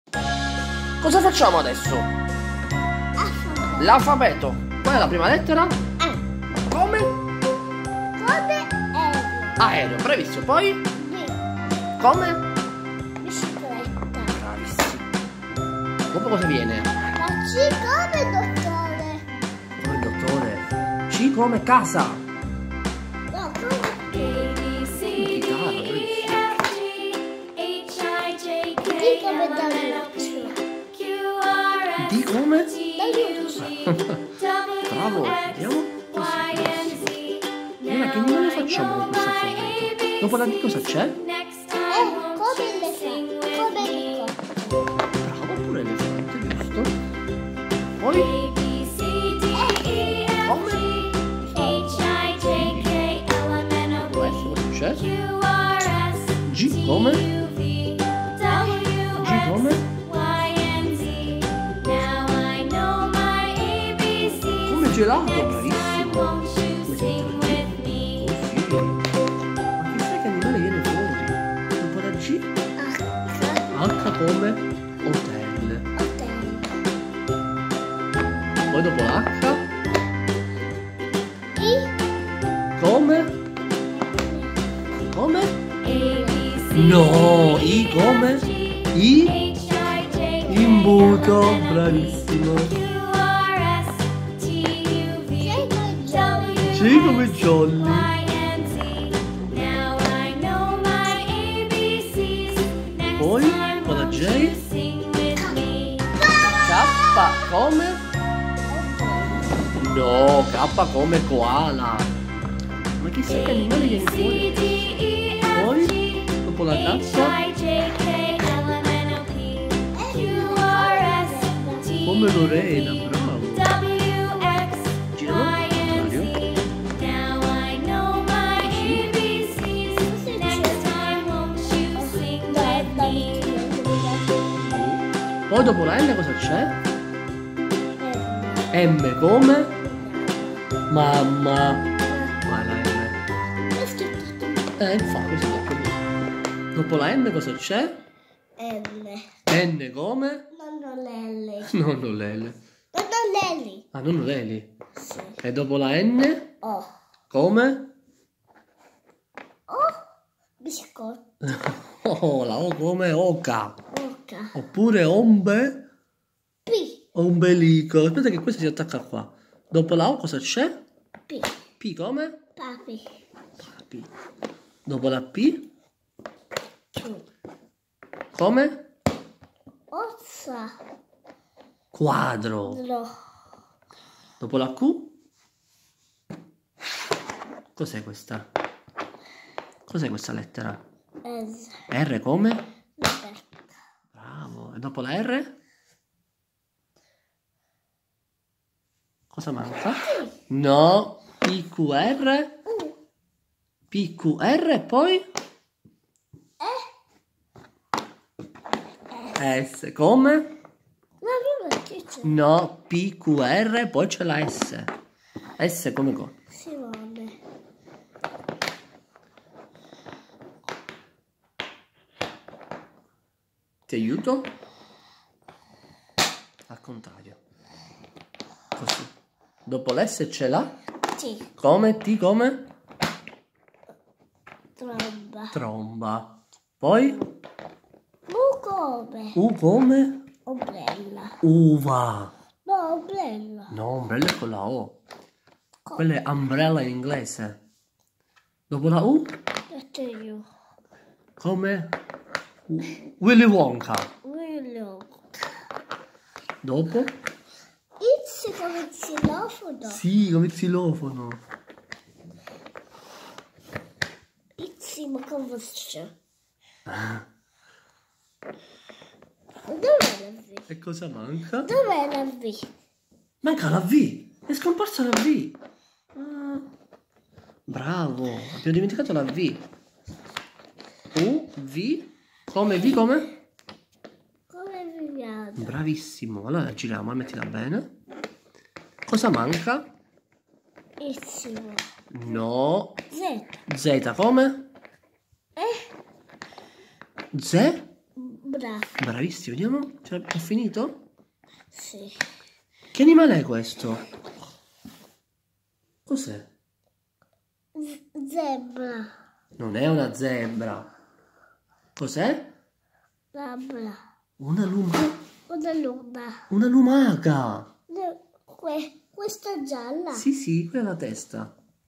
Simone insegna l'alfabeto
Il piccolo Simone insegna l'alfabeto ai bimbi